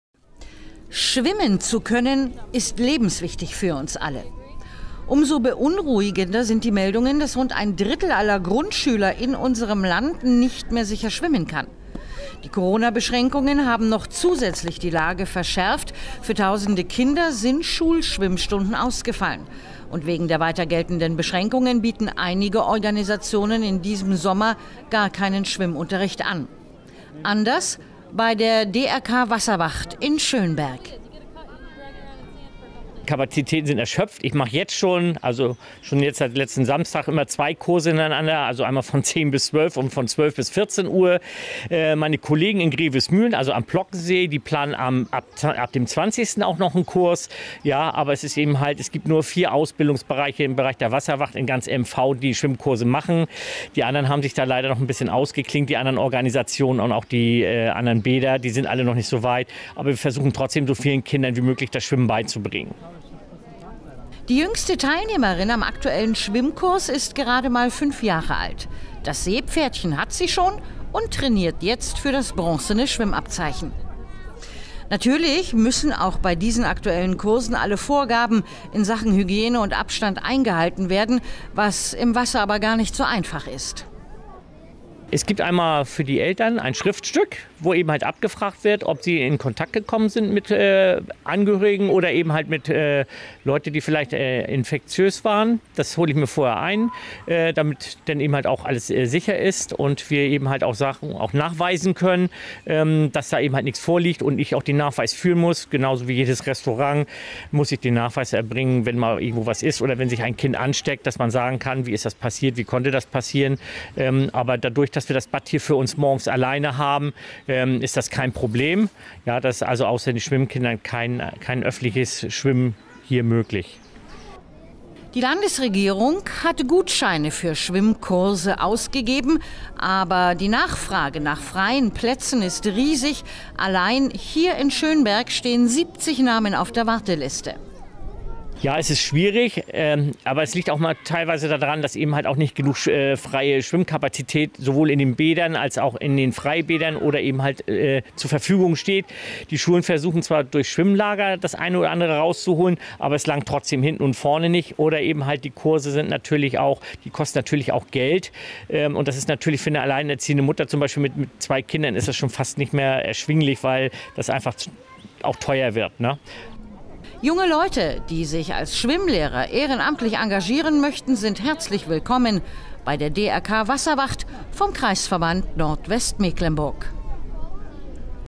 Schwimmen lernen ist lebenswichtig
Immer weniger Grundschüler in unserem Land können sicher schwimmen. Wegen der Corona-Beschränkungen fallen in diesem Sommer viele Schwimmkurse aus. Die DRK Wasserwacht in Schönberg versucht dagegen so vielen Kindern wie möglich, das Schwimmen beizubringen. Wir haben am 13. Juli das Naturbad Schönberg besucht.